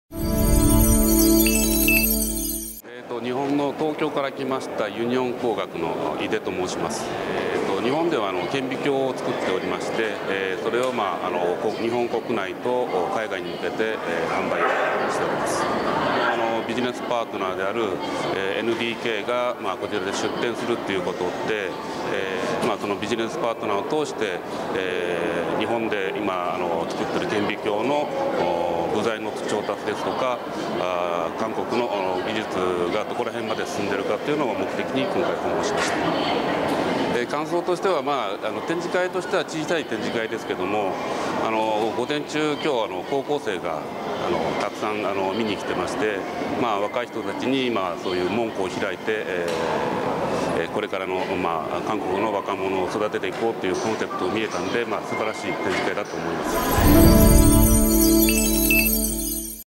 [IT EXPO 2011 영상] 일본의 유니온광학 "한국의 기술 알아보고자 참관"
'전자산업의 미래와 기업이 만나다'란 주제로 '2011 국제전자엑스포'가 9월 26일부터 28일까지 3일간에 걸쳐 열리고 있는 가운데, 일본, 포르투갈, 스페인, 인도 등 다양한 국가로부터 많은 해외 바이어들이 전시회를 찾았다.

일본 유니온광학의 Yuji Ide 해외사업부 부장과 인터뷰를 진행했다.

Q. 유니온광학은 어떤 회사인가?

일본 도쿄에서 온 Yuji Ide라고 합니다. 저희 유니온광학은 현미경을 제조해 일본 국내 및 해외에 판매하고 있습니다.

Q. 이번 '2011 국제전자엑스포(IT EXPO 2011)'에 참가한 목적은?

한국의 비즈니스 파트너인 엔디케이가 참가한다는 것이 계기가 되었습니다. 올해 처음 개최되는 이 전시회에서 한국의 발전된 기술을 알아보고자 방문했습니다.

Q. '2011 국제전자엑스포(IT EXPO 2011)'에 대한 느낌은?

규모는 작은 전시회지만 많은 학생들이 관람을 온 것이 인상적이었습니다. 젊은이들에게 문호를 개방해 인재를 육성하려 한다는 점에서 훌륭한 전시회라고 생각합니다.